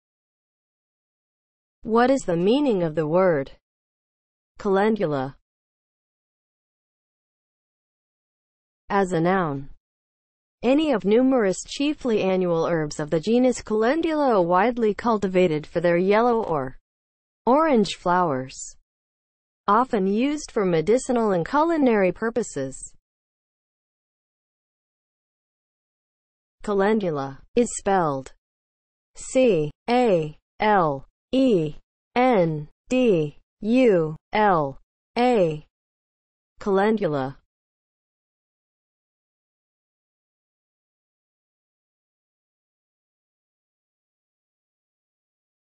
What is the meaning of the word CALENDULA?
Definition and spelling of the word CALENDULA.

(00:00) Intro
(00:07) As a noun
(00:24) Spelling

As a noun (00:07):
• any of numerous chiefly annual herbs of the genus Calendula widely cultivated for their
  yellow or orange flowers; often used for medicinal and culinary purposes